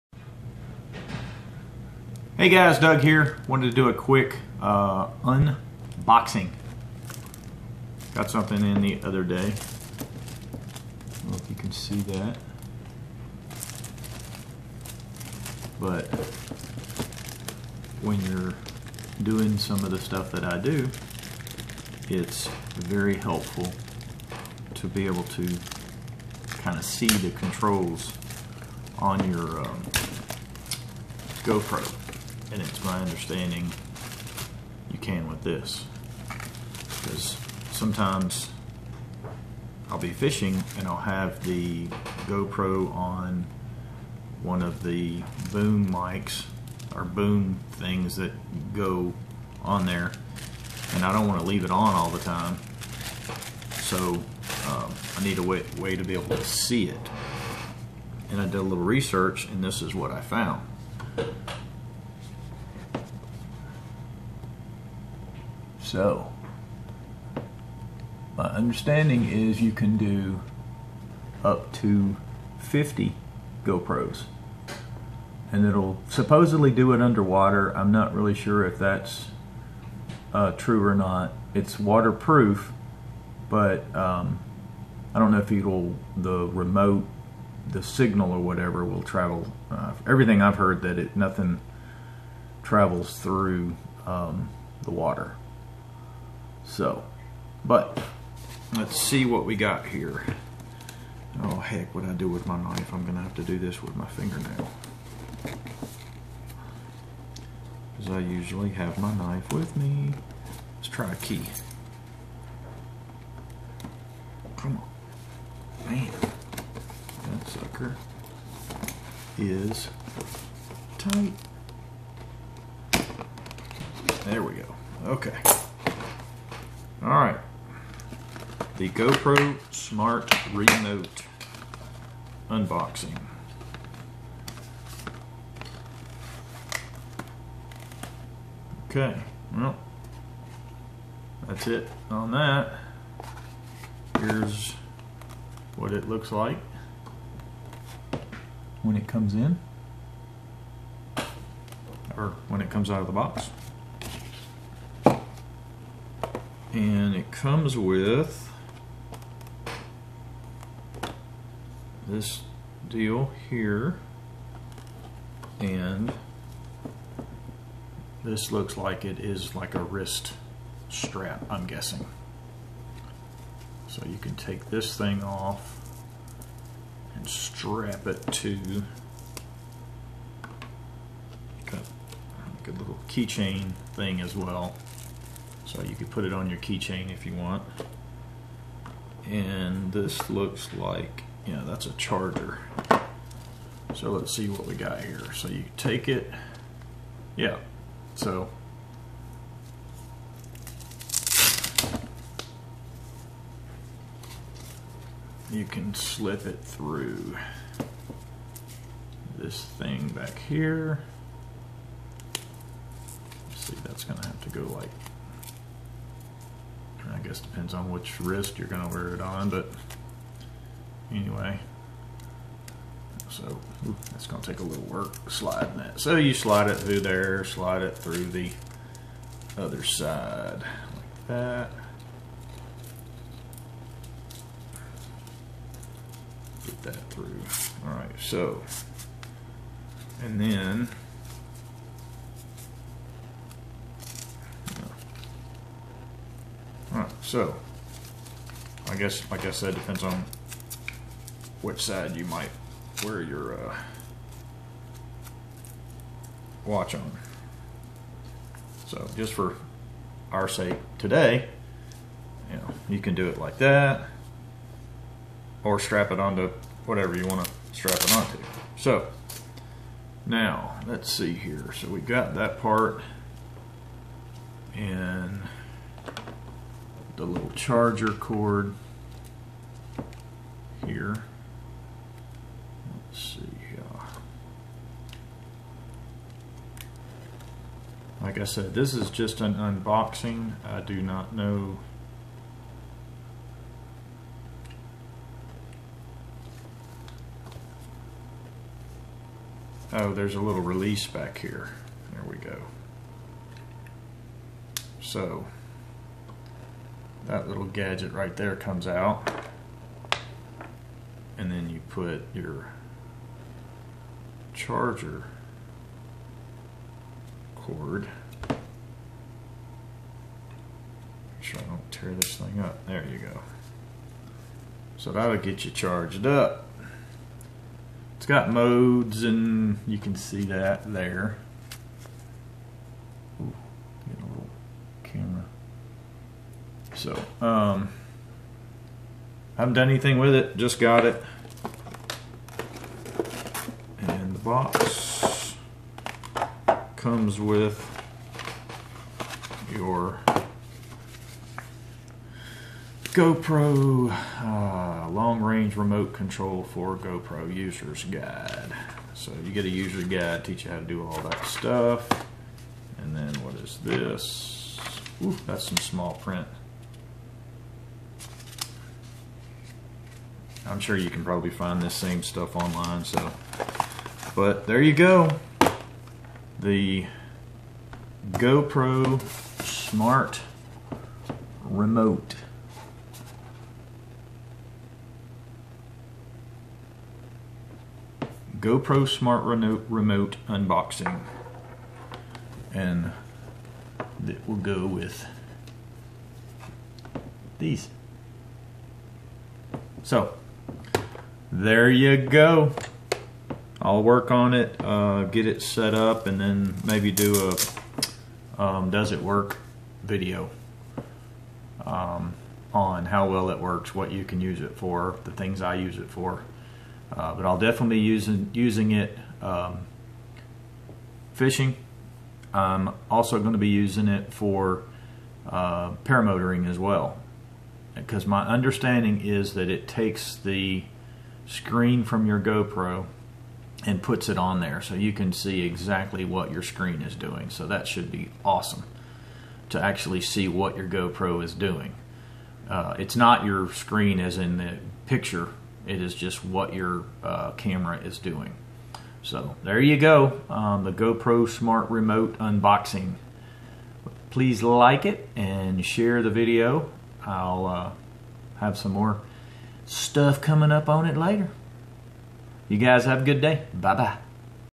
Go Pro smart remote unboxing
Short video of me unboxing the GoPro Smart remote.  Should work with my HERO 5 & 6.

HERE IS THE AMAZON LINK IF YOU WOULD LIKE TO PICK UP ONE FOR YOURSELF